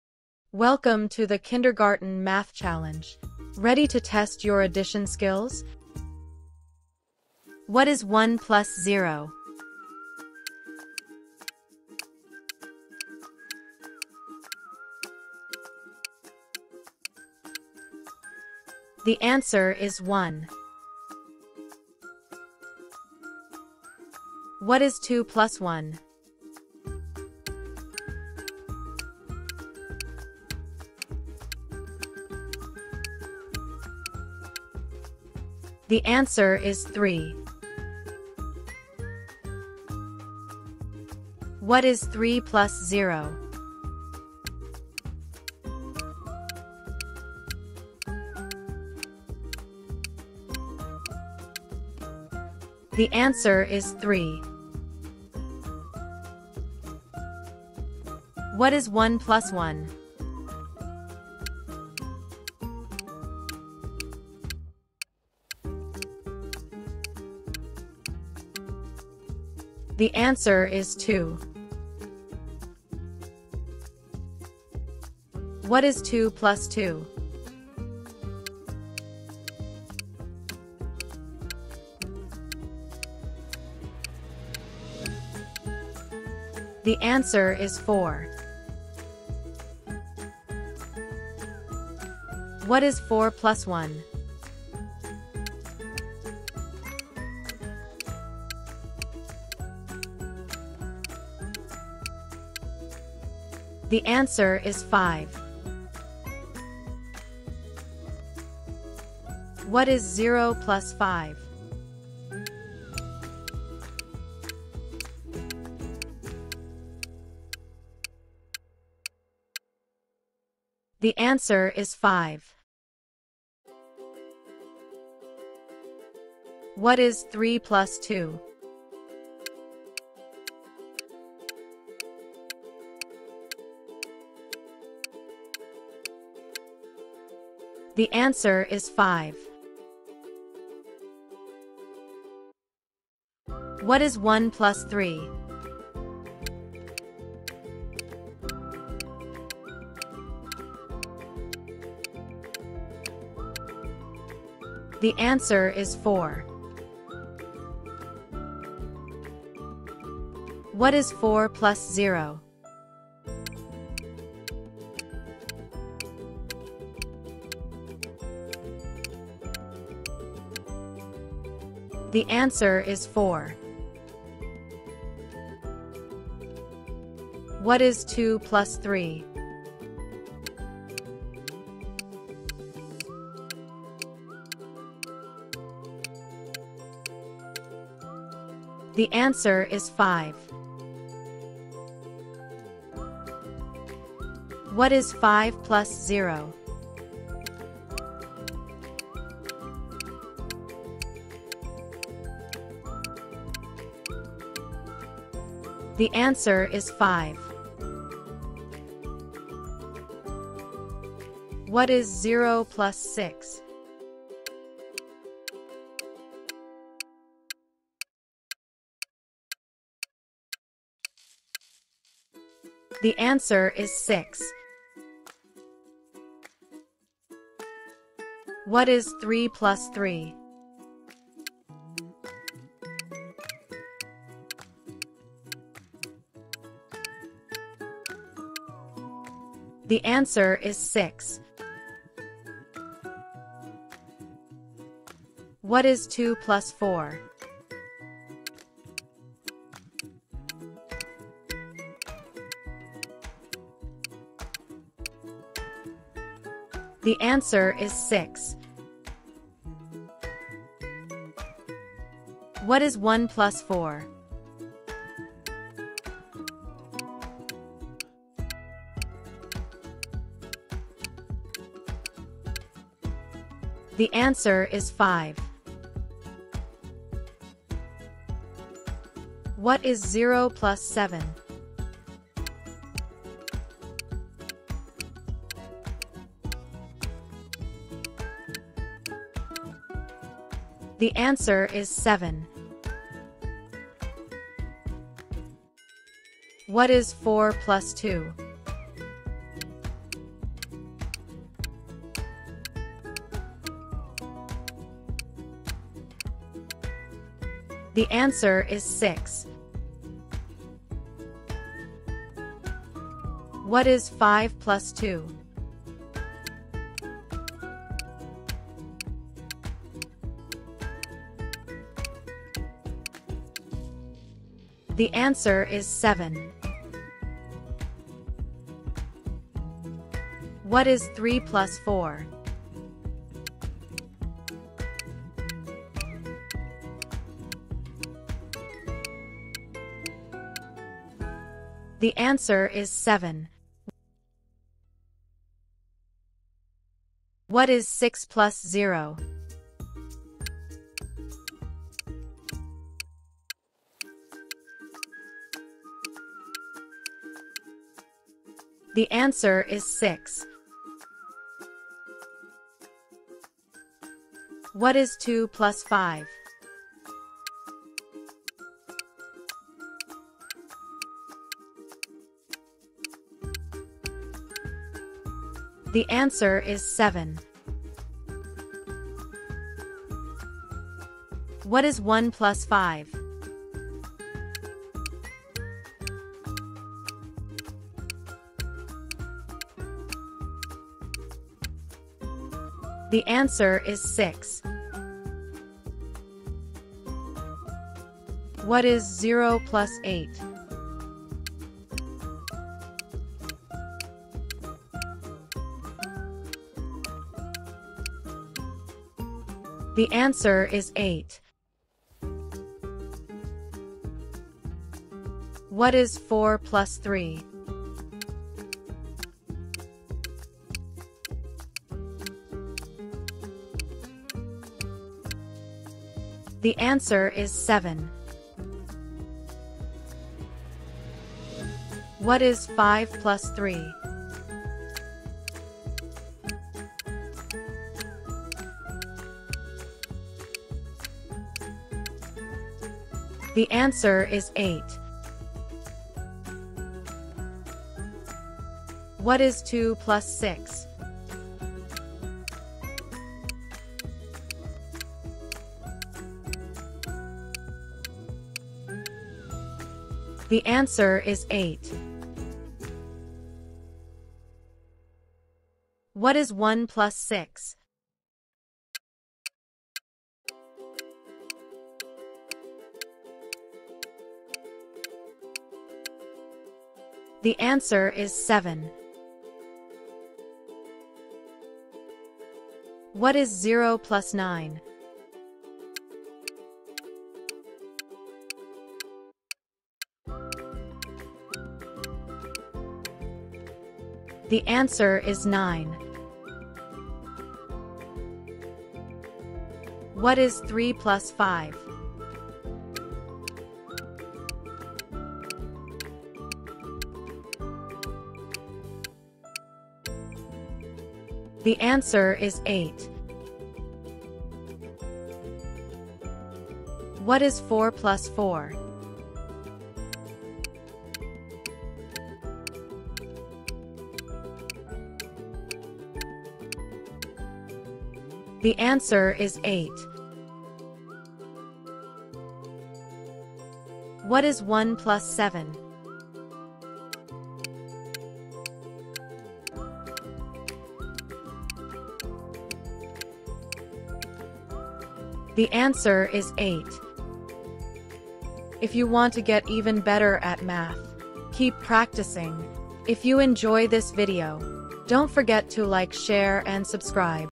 Addition Quiz for Kids ➕🎉 | Kindergarten Math Practice
Welcome to our Addition quiz for kids! ➕✨
This video is specially designed for Kindergarten learners to build early math skills through simple and fun addition problems. With clear questions and answers, children can easily follow along and practice adding numbers step by step.

✨ In this Addition quiz for kids, children will:
✔️ Practice addition with numbers from 0 to 10
✔️ Strengthen counting and number recognition
✔️ Build confidence in solving basic math problems
✔️ Enjoy learning through an interactive quiz format

🎯 Perfect for:

Kindergarten students

Homeschool practice

Classroom math warm-ups

After-school learning

🎮 Play, pause, and answer together to make addition learning simple and fun!